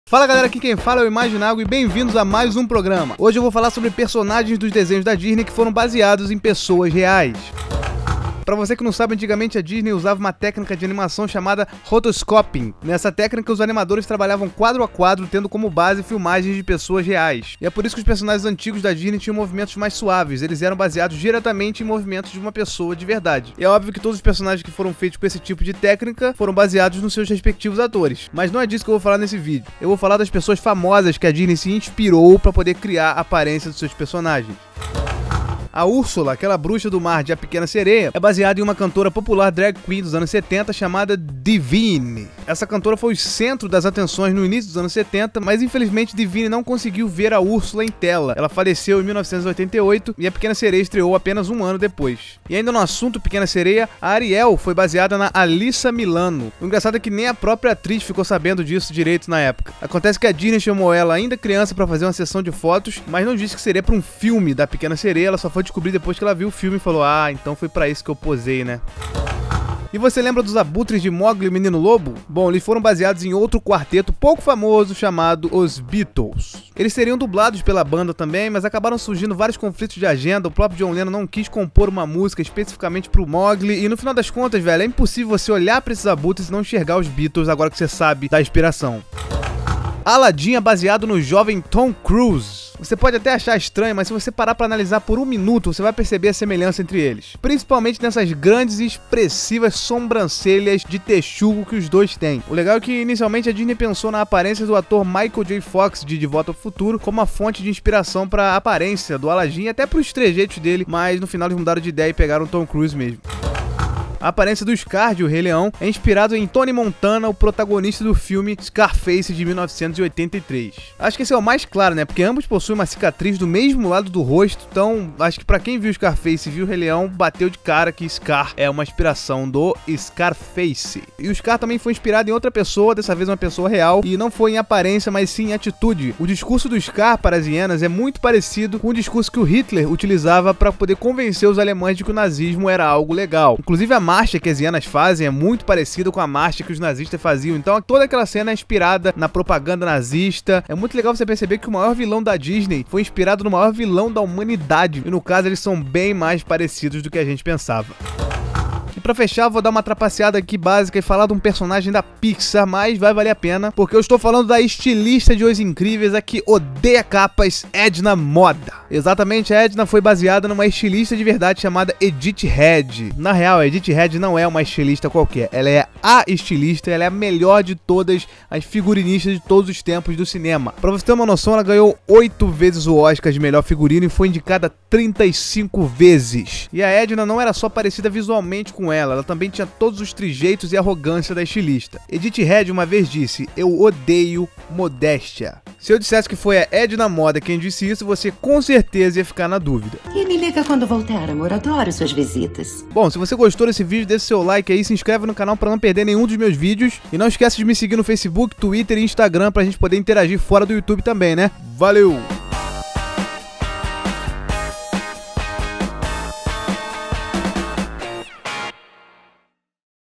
Personagens da Disney inspirados em pessoas REAIS
Vários personagens dos desenhos da Disney foram inspirados em pessoas famosas e reais! Nesse vídeo, eu falo de alguns!

Me segue ai! =)

~~Direitos autorais~~